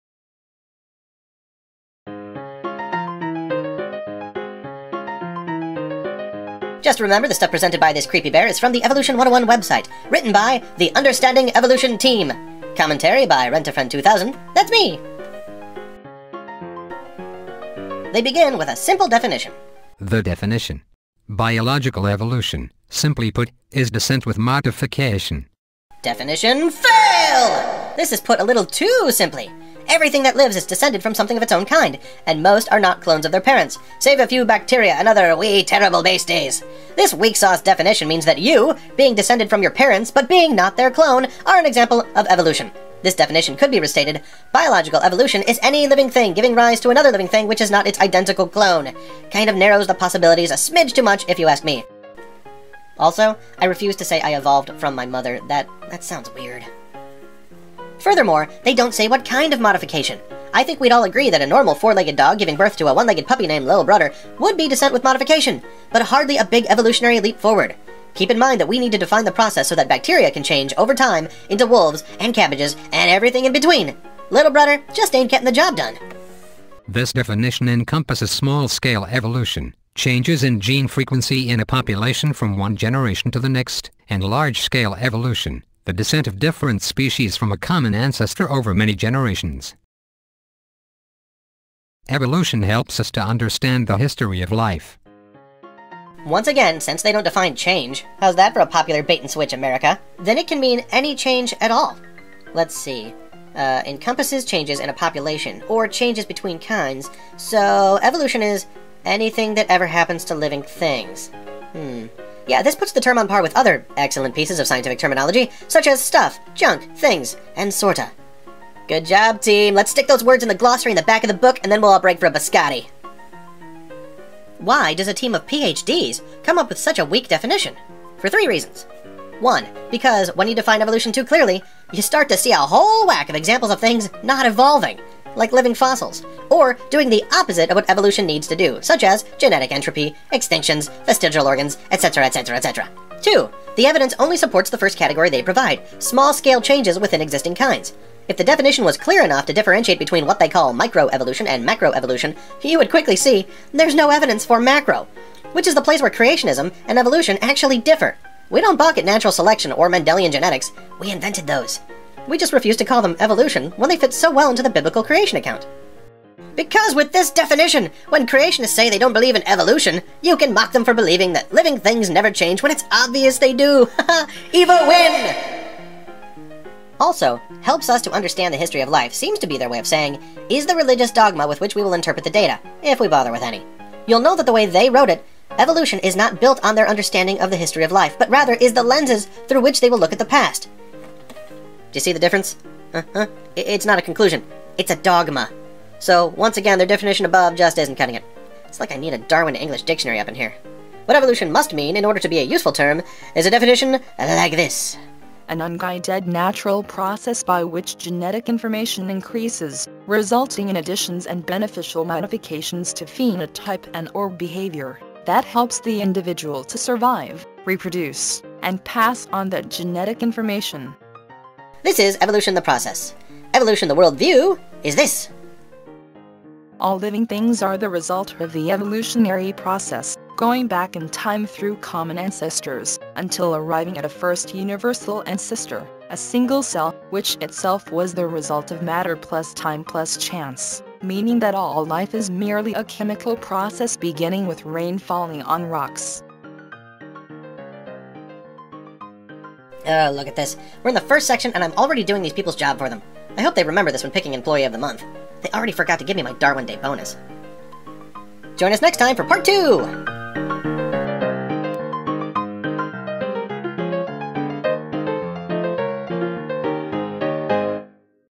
Evolution 101-part 1: Definition FAIL
Berkley fails at their first attempt to define just what "evolution" is. Rent a Friend 2000 explains why their definition is a major fail, and offers a definition which would actually be useful, especially as it makes clear the difference between Creation and Evolution. You have to admit that, if those two words don't mean different things, then that Ken Ham Vs Bill Nye debate becomes a lot harder to follow.

The definition
Biological evolution, simply put, is descent with modification.

Definition FAIL. This is put a little TOO simply. Everything that lives is descended from something of its own kind, and most are not clones of their parents– save a few bacteria and other wee terrible beasties. This weak sauce definition means that you, being descended from your parents (but being not their clone) are an example of evolution. This definition could be restated, “Biological Evolution is any living thing giving rise to another living thing which is not its identical clone.” Kind of narrows the possibilities a smidge TOO much if you ask me. Also, I refuse to say I “evolved from my mother.” That sounds weird.

Furthermore, they don’t say what KIND of modification. I think we’d all agree that a normal, four legged dog giving birth to a one legged puppy named Lil’ Brudder would be decent with modification, but hardly a big evolutionary leap forward. Keep in mind that we need to define the process so that bacteria can change, over time, into wolves and cabbages and everything in between. Lil’ Brudder just ain’t getting the job done.

This definition encompasses small-scale evolution (changes in gene frequency in a population from one generation to the next) and large-scale evolution (the descent of different species from a common ancestor over many generations). Evolution helps us to understand the history of life.

Once again, since they don’t define “change” (how’s that for a popular bait and switch, America?) then it can mean ANY change at all. Let’s see- encompasses changes in a population, or changes between kinds. So…. Evolution is anything that ever happens to living things. This puts the term on par with other excellent pieces of scientific terminology, such as “Stuff,” “Junk,” “Things,” and “Sorta.”

Good job team! Let’s stick those words in the glossary in the back of the book, and then we’ll all break for a biscotti.
Why does a team of PhD’s come up with such a weak definition? For three reasons:

1. Because when you define evolution too clearly, you start to see a whole WACK of examples of things not evolving (like living fossils), or doing the opposite of what evolution needs to do (such as genetic entropy, extinctions, vestigial organs, etc).

2. The evidence ONLY supports the FIRST category they provide- small scale changes within existing kinds. If the definition was clear enough to differentiate between what they call micro and macro evolution, you would quickly see that there is NO evidence for Macro, which is the place where Creationism and Evolution actually differ. We don’t balk at Natural Selection or Mendelian genetics. We invented those. We just refuse to call them “Evolution” when they fit so well into the Biblical Creation account.

3. Because with this definition, when Creationists say they don’t believe in “evolution,” you can mock  them for believing that living things never change when it’s OBVIOUS that they do. Evo WIN!

It’s like I need a Darwin to English dictionary up in here. What Evolution MUST mean in order to be a useful term is a definition like this:  “An unguided natural process by which genetic information increases, resulting in additions and beneficial modifications to phenotype and/or behavior that helps the individual to survive, reproduce, and pass on that genetic information.”

This is evolution the process. Evolution the worldview is the belief that  “All living things are the result of the evolutionary process, going back in time through common ancestors until arriving at a first universal ancestor, a single cell, which itself was the result of matter plus time plus chance, meaning that all life is merely a chemical process beginning with rain falling on rocks.”

Look at this, we’re in the first section and I’m already doing these people’s job for them. I hope they remember this when picking the employee of the month. They already forgot to give me my Darwin Day bonus.